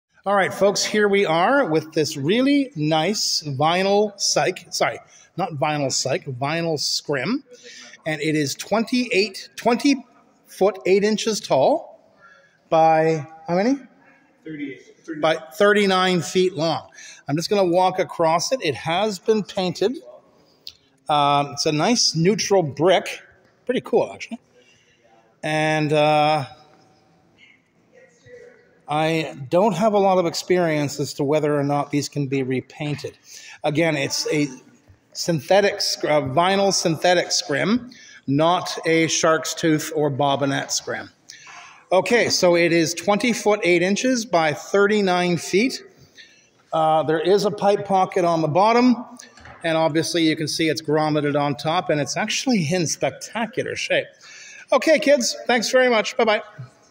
vinyl scrim 20 foot 8 inches x 30 feet
Grommeted on top with pipe pocket on bottom. 20 foot 8 inches x 30 feet. black brick image.